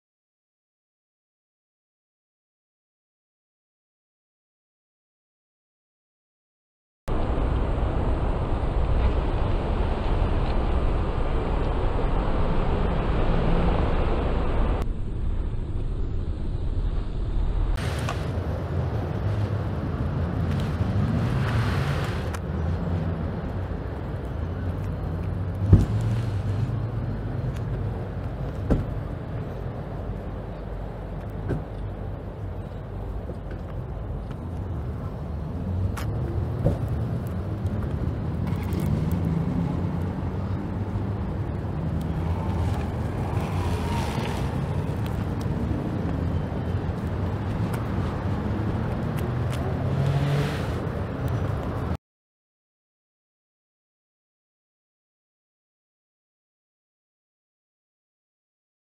Billboard LED bicycle three wheeled trailer with 3S steering - NEWLY PATENTED
Balkan Innovation Center have developed and tested number of innovations and patents based on high energy efficient synergy of renewable energy sources and environment friendly road and water vehicles.